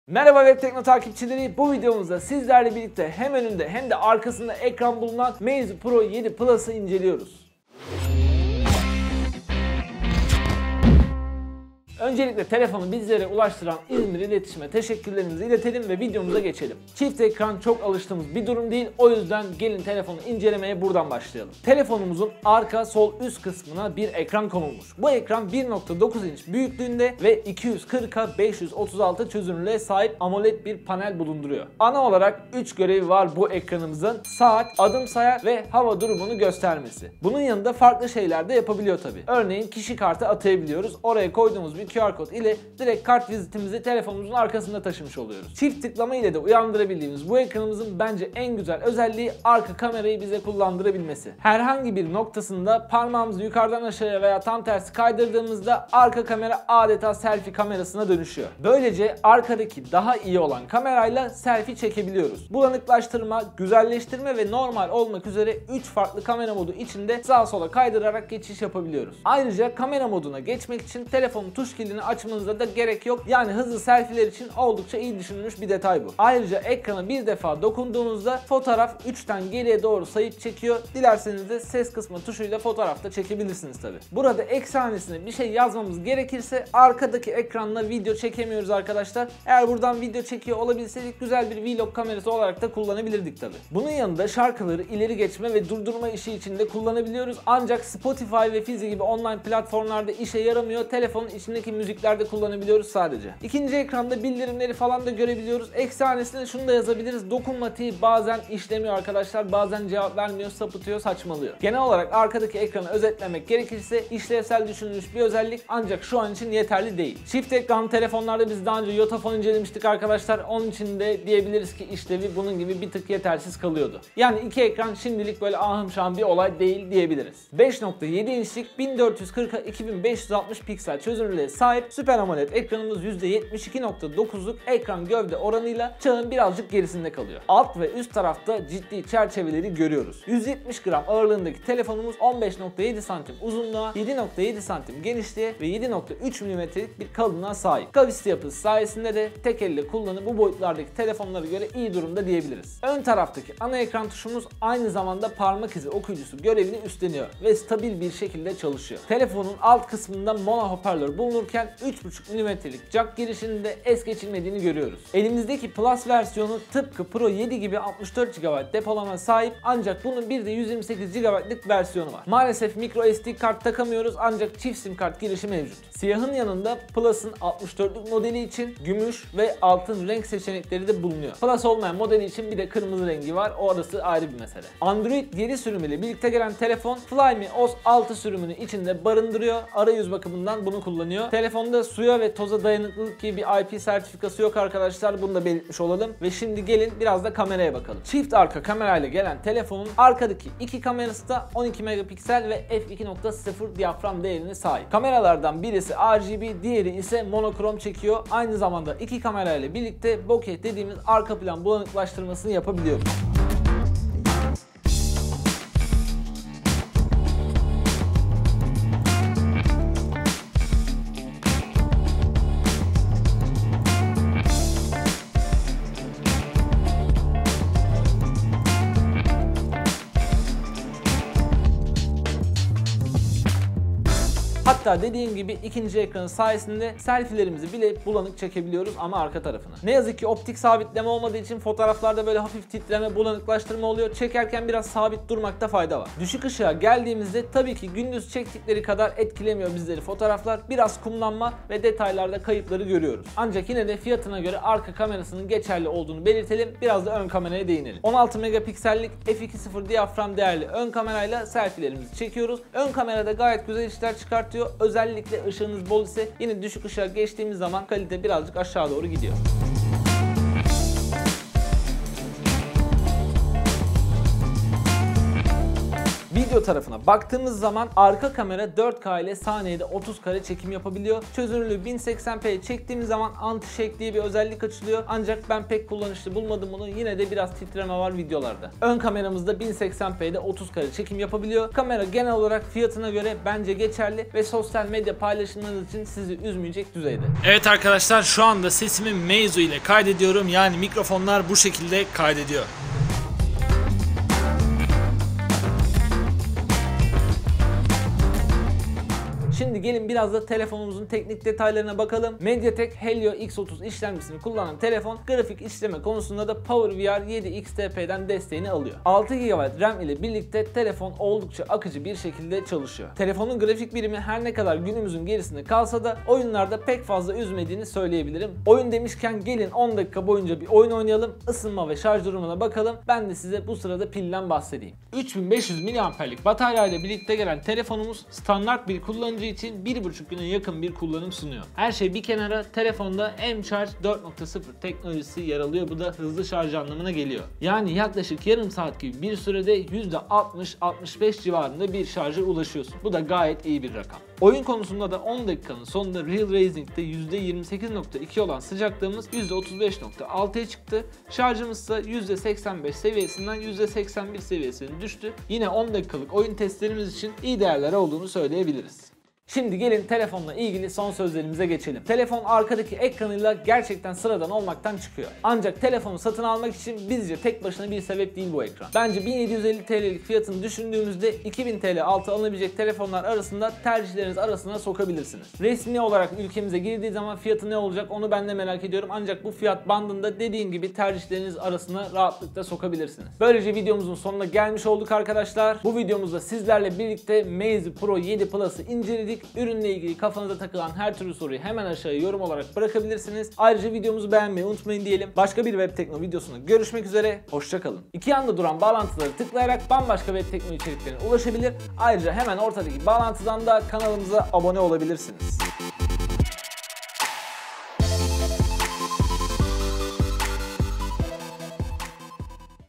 Çift Ekranlı 10 Çekirdekli 3 Kameralı Meizu Pro 7 Plus İncelemesi
Çift Ekranlı 3 Kameralı 10 İşlemcili Meizu Pro 7 Plus İncelemesi (Kuş Sütü Nerede?)

Merhaba Webtekno takipçileri bu videomuzda sizlerle birlikte hem önünde hem de arkasında ekranı bulunan telefon Meizu Pro 7 Plus’ı inceliyoruz.

Telefonumuzun arka sol üst kısmına bir ekran konulmuş. Bu ekran 1.9 inç büyüklüğünde ve 240’a 536 çözünürlüğe sahip amoled bir panel bulunduruyor. Bu kadar küçük bir ekran için iyi bir çözünürlük diyebiliriz. Peki bu ekran neler yapabiliyor. Ana 3 görevi var bu ekranın. Saat, adımsayar ve hava durumunu göstermesi. Bunun yanında farklı şeylerde yapılabiliyor tabi. Örneğin kişi kartı atayabiliyoruz. Oraya koyduğumuz bir qr kod ile direk kartvizitimizi telefonumuzda taşımış gibi oluyoruz.  Çift tıklama ile uyandırabildiğimiz bu ikinci ekranımızın bence en güzel özelliği kamerayı bize kullandırması. Herhangi bir noktasında parmağımızı yukarıdan aşağıya veya tam tersi kaydırdığımızda arka kamera adeta selfie kamerasına dönüşüyor. Yani arkadaki ekranla kendimizi görebiliyoruz.Böylece arkadaki daha iyi olan kamerayla selfie çekebiliyoruz. Bulanıklaştırma, güzelleştirme ve normal olmak üzere 3 farklı kamera modu içinde de sağa sola kaydırarak geçiş yapabiliyoruz. Bu menüden çıkmak için tekrar parmağımızı yukarı veya aşağı kaydırmamız gerekli. Ayrıca kamera moduna geçmek için telefonun tuş kilidini açmanıza gerek yok. Yani hızlı selfieler için oldukça iyi düşünülmüş bir detay bu. Ayrıca ekrana bir defa dokunduğunuzda fotoğraf 3’ten geriye sayıp çekiyor. Burada eksi hanesine bir şey yazmak gerekirse keşke video modu da ekleselermiş diyebiliriz. O zamangüzel bir vlog kamerasına da dönüşmüş olurdu. Bunun yanında şarkıları ileri geçme ve durdurma işi içinde kullanabiliriz. Ancak Spotify fizy gibi online platformlarda işlemiyor maalesef. Sadece telefonun kendi içindeki oynatıcıda çalışıyor. Bu da biraz anlamsız kılmış bu özelliği.  İkinci ekran bazen dokunmatik anlamında tepki vermeyebiliyor. Genel olarak arka ekranı özetlediğimizde işlevsel olmaya meyilli ancak şimdilik bana göre yetersiz kalmış.

-devamı için videoyu izleyebilirsiniz :)-